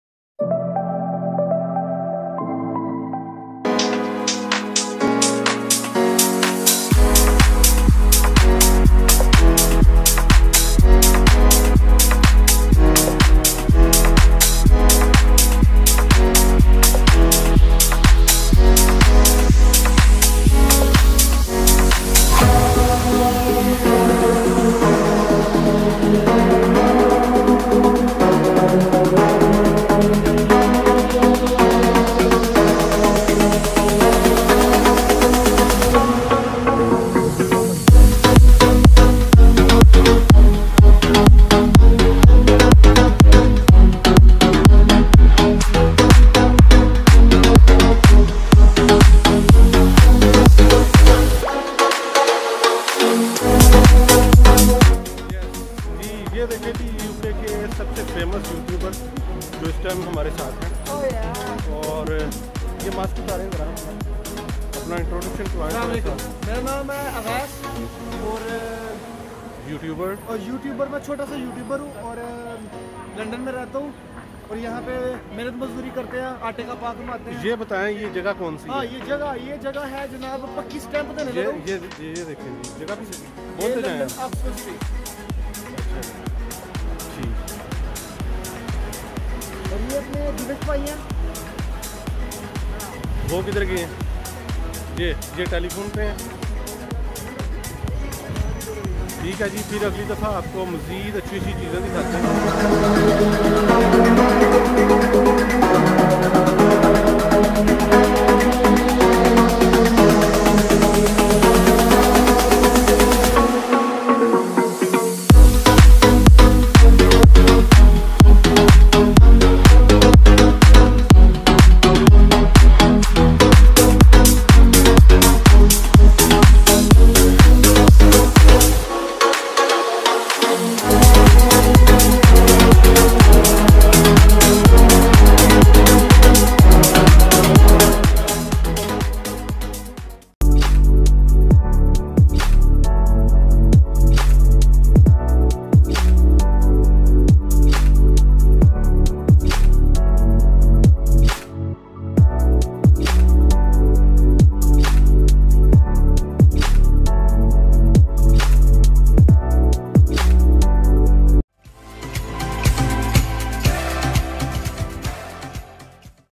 London Oxford Street | London Walk 2020 | Walking London Oxford Street
📹📺 Video Edit & Directed by :
🔖Khurram Shehzad
Official Channel @Traveltrekker✔

-This video is also for teaching purposes for refugees and against the human trafficking.

This London walking your covers the full length of Baker street until we reach Oxford Street. 221B Baker Street is the London address of the fictional detective Sherlock Holmes. I pass by this address at the beginning of the video, but unfortunately due to traffic from both sides, it was not clear to see.  At this address currently there is a The Sherlock Holmes Museum. It is a privately run museum in London, England, dedicated to the famous fictional detective Sherlock Holmes. It is the world's first museum dedicated to the literary character Sherlock Holmes